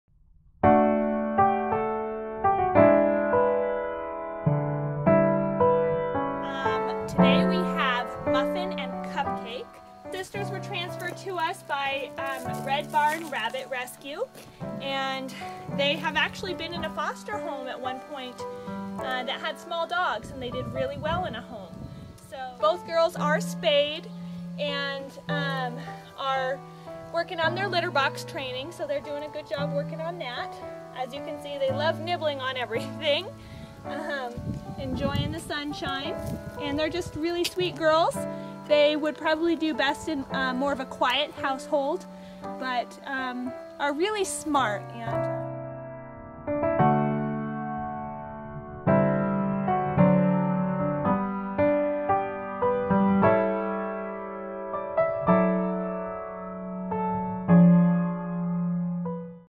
Muffin & Cupcake
Meet Muffin & Cupcake, New Zealand-Netherland dwarf bunnies.